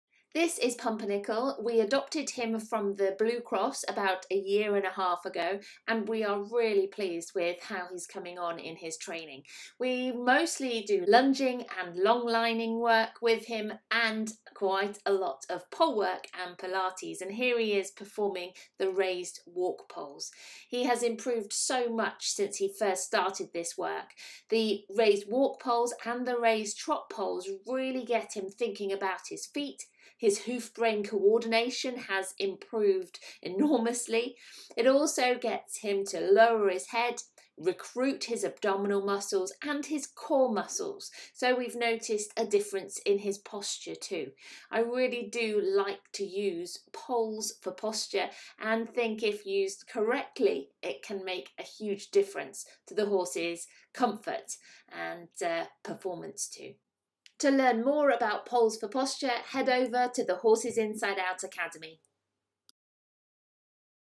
Horse Poles for Posture - How to do it!
Our Blue cross adopted pony Pumpernickel, demonstrates his skill and agility during a Poles for Posture, polework session.